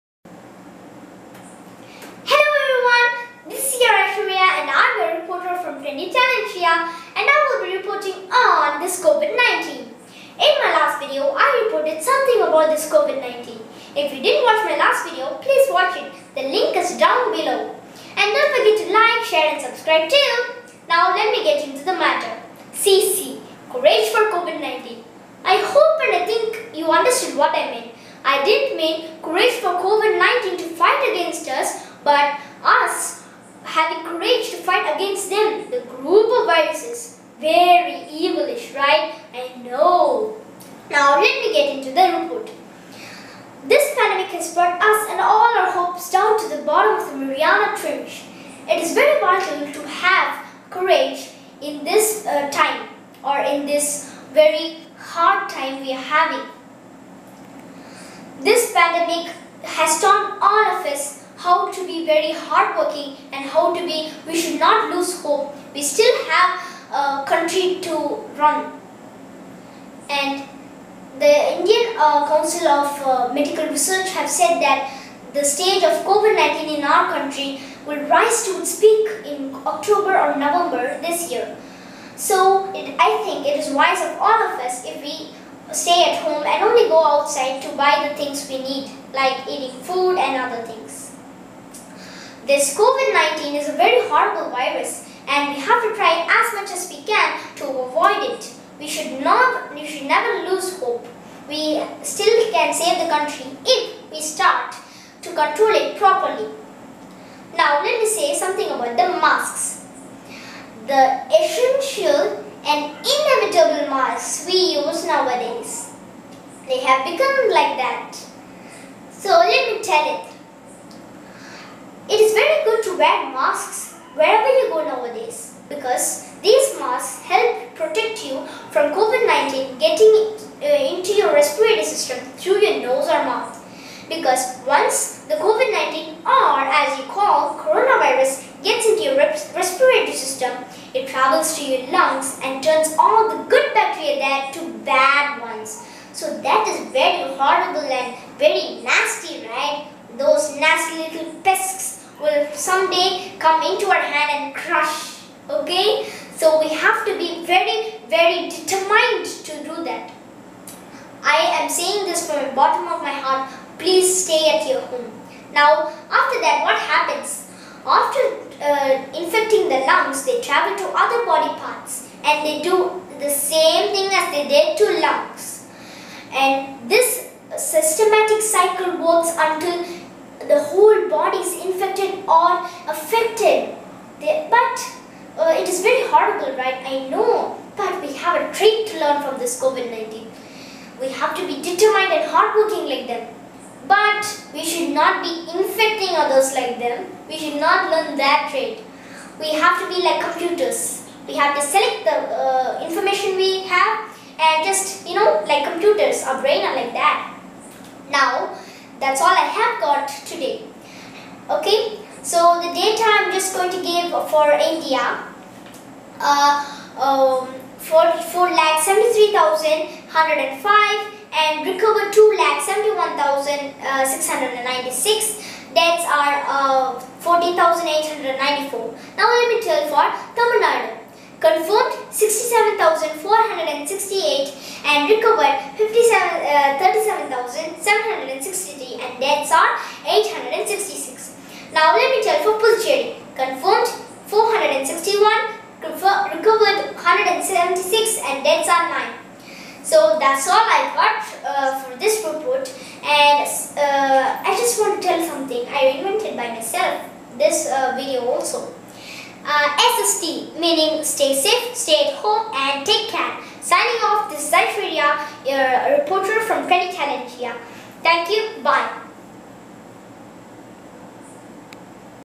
Covid 19 Hope To Live
In this video, I have reported on how to be courageous and hopeful to fight against the COVID - 19. Stay safe, Stay at home, and Take care.